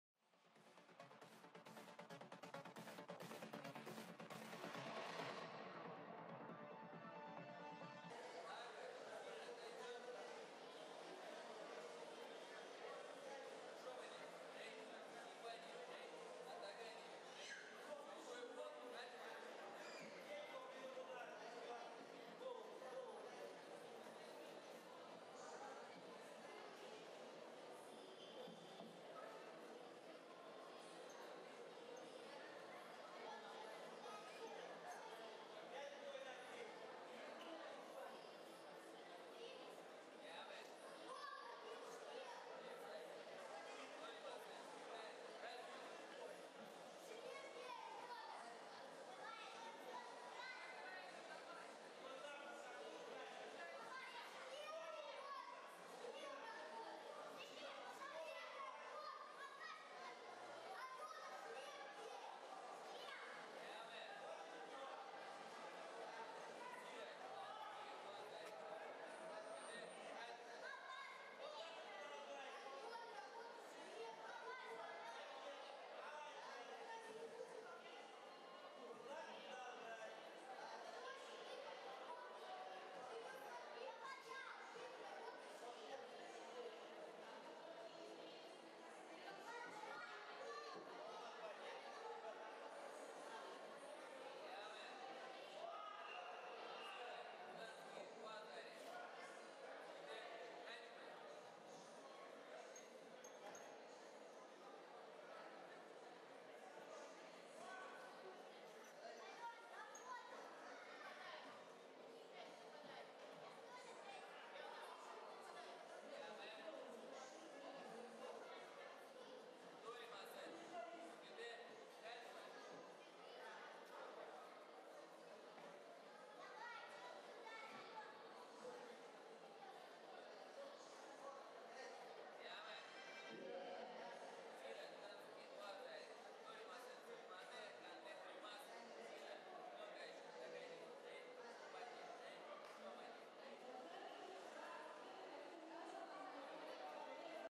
Косики Каратэ Кумитэ. Пащенко Д. - Игнатьева А. Фестиваль Контактного Каратэ. 25.03.2015. Полуфинал
Фестиваль спортивных единоборств по - Спортивному Контактному Каратэ, г. Новокузнецк.
25 марта 2015 г.
Косики Каратэ / Koshiki Karate / 硬式空手道

Festival of martial arts for - Sports Contact Karate, Novokuznetsk.
Koshiki Karate / Косики Каратэ / 硬式空手道